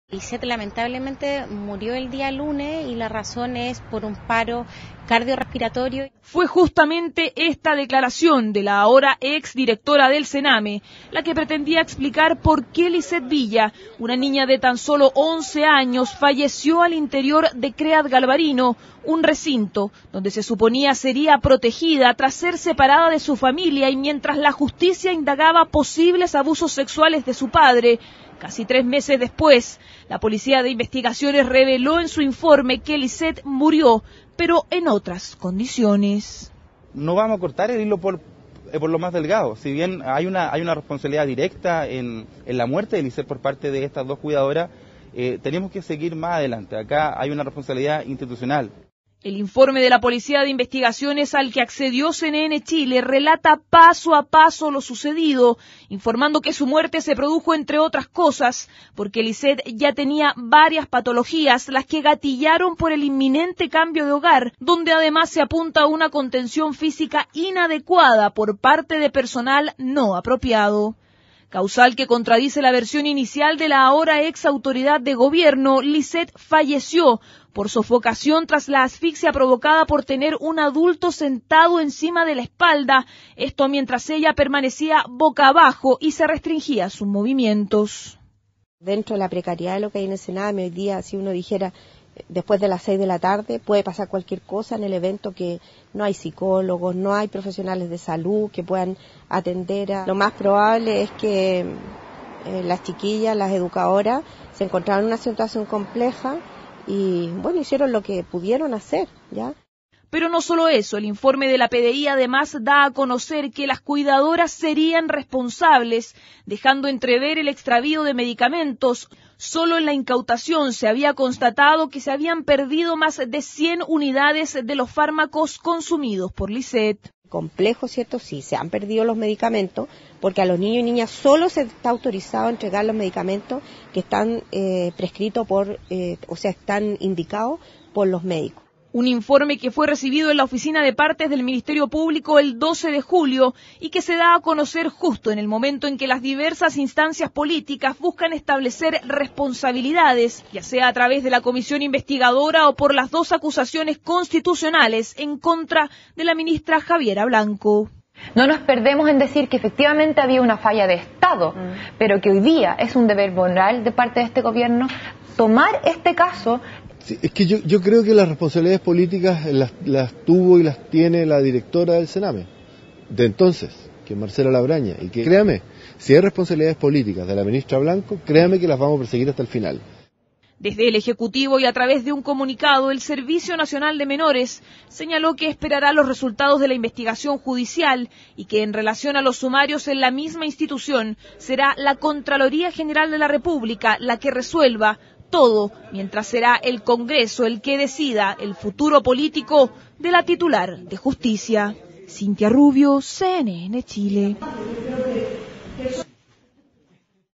PDI responsabiliza a cuidadoras por muerte de menor en recinto del Sename
El escrito señala que pudo haber intervención de terceros, ya sea por acción u omisión.